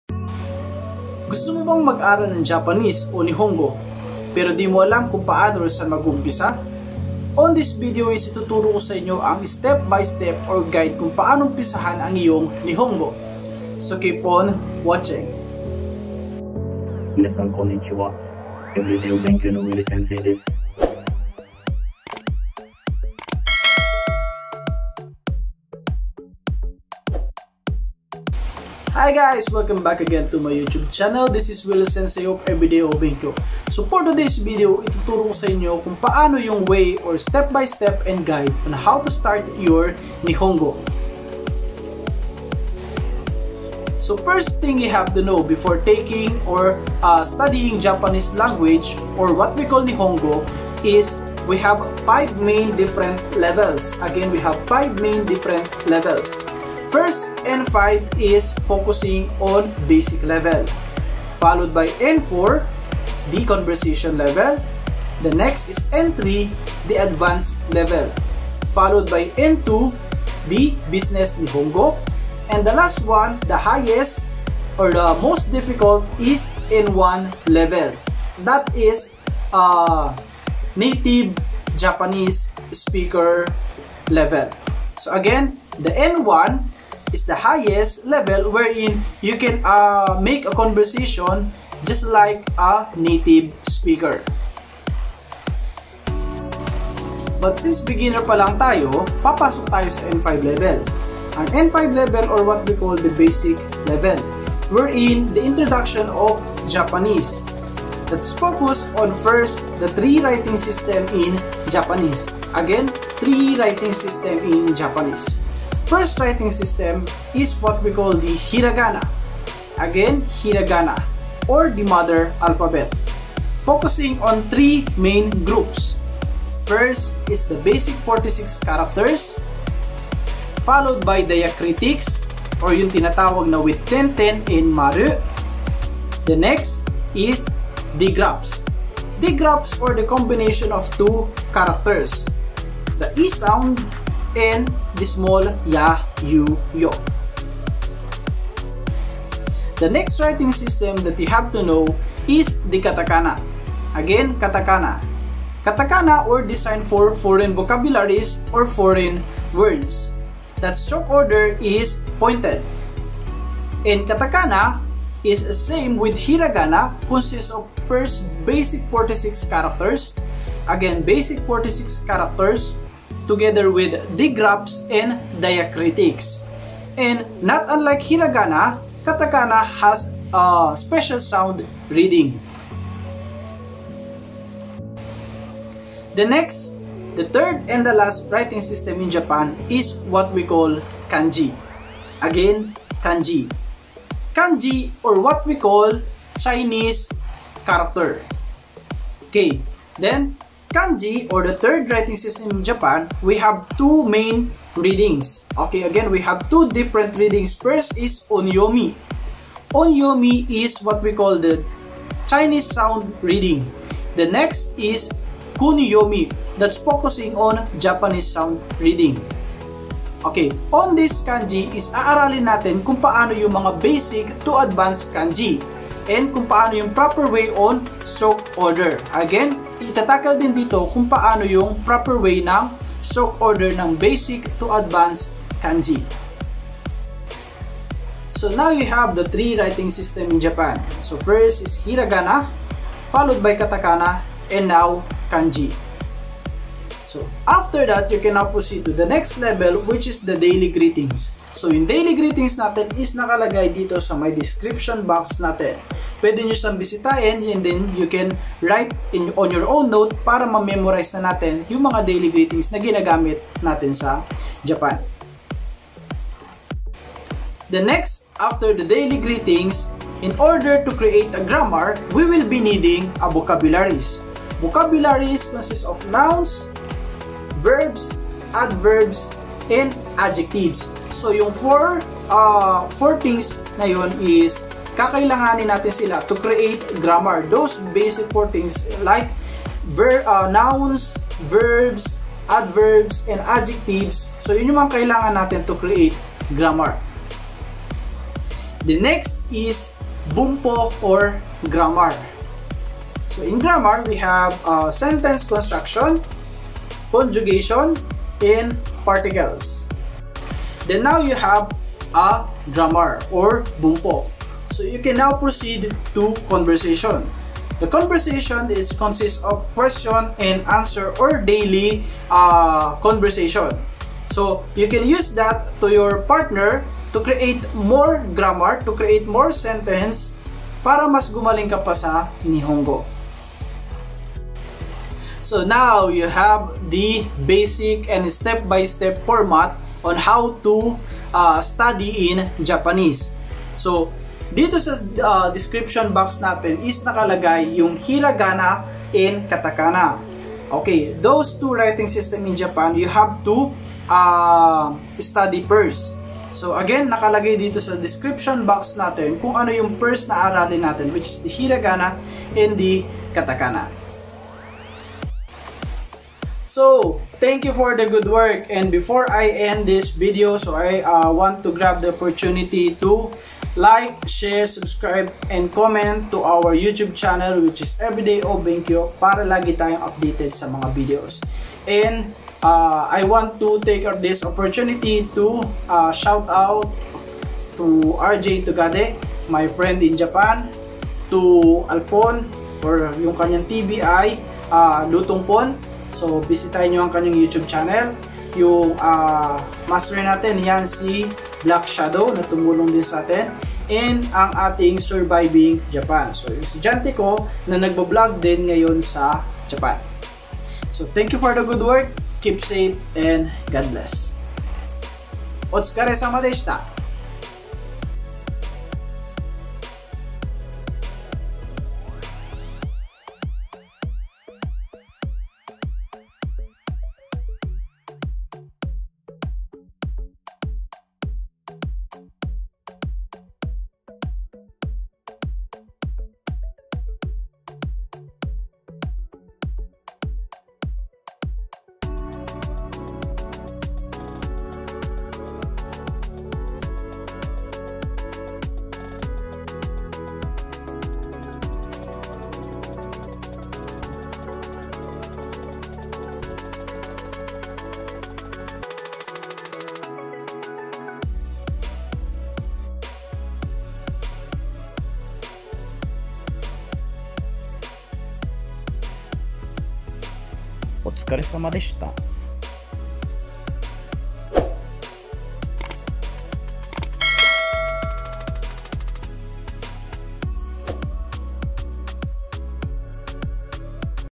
STEP by STEP GUIDE BEFORE LEARNING JAPANESE (Learn with WIL SENSEI)
Taking a Japanese Language Lesson is not easy, so you have to know this things before studying for you to have a guide on what and where to start.